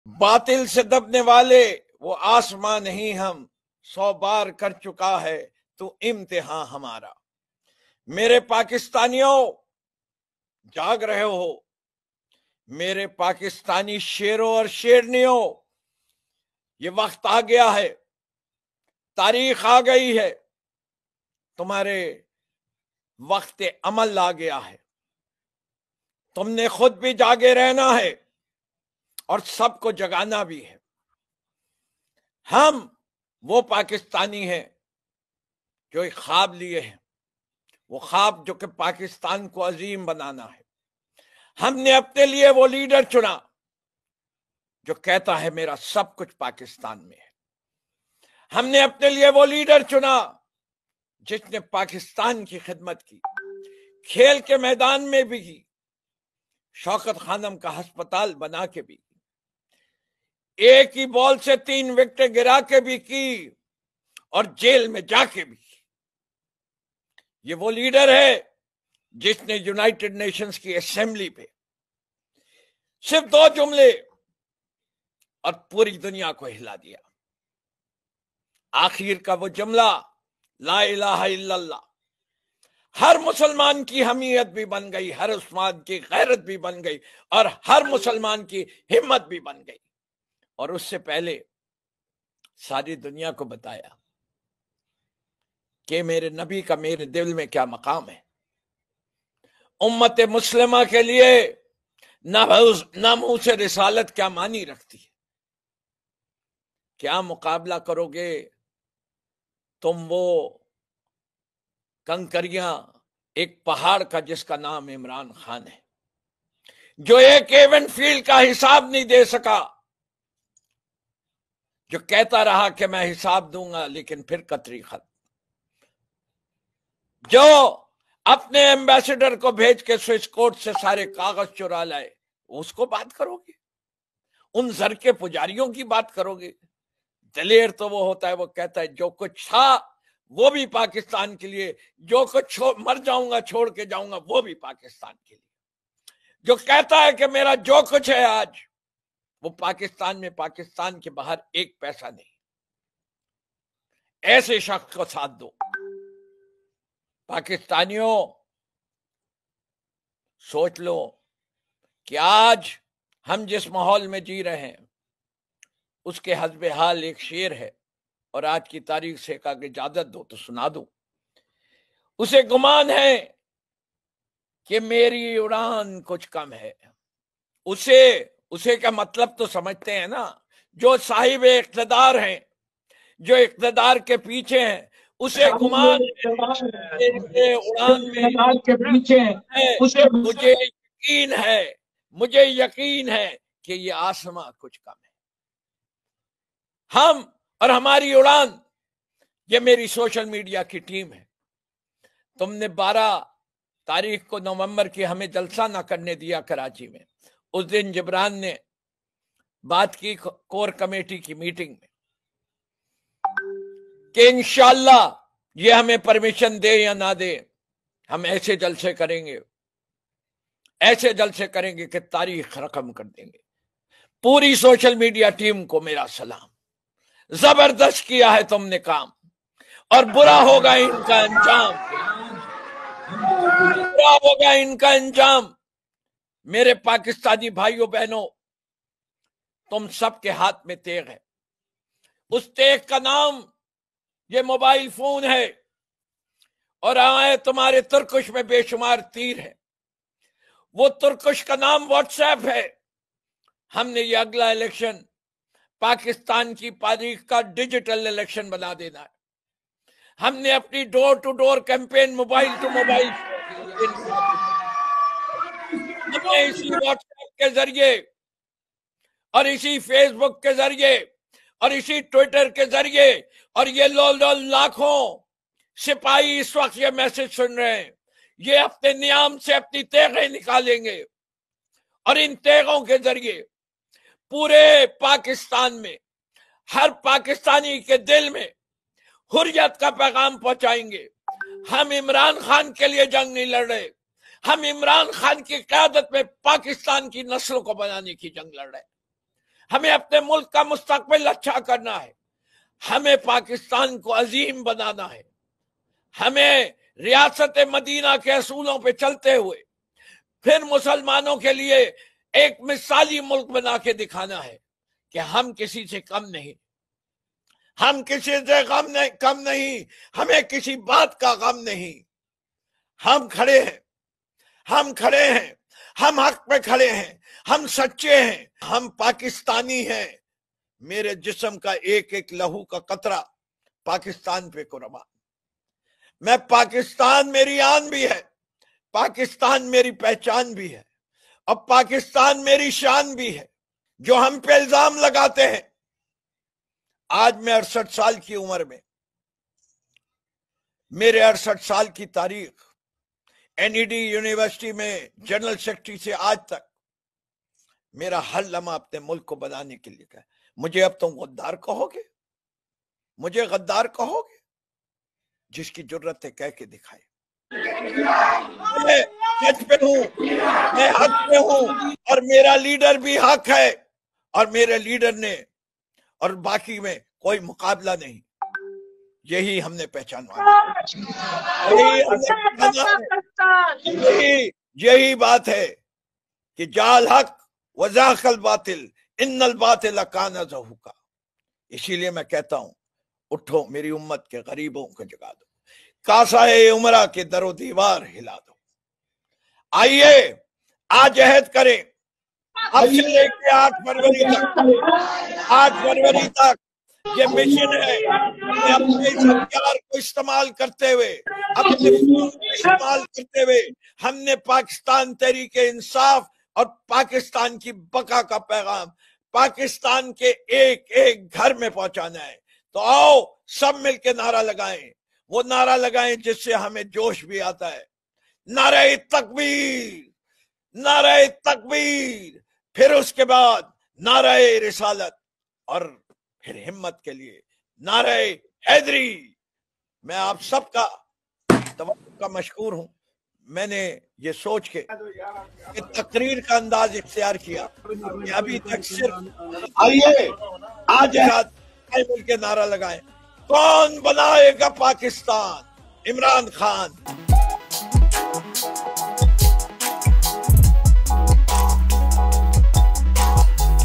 PTI Leader Firdous Shamim Naqvi Furious Address In PTI Online Jalsa - CurrentNN
| News Updates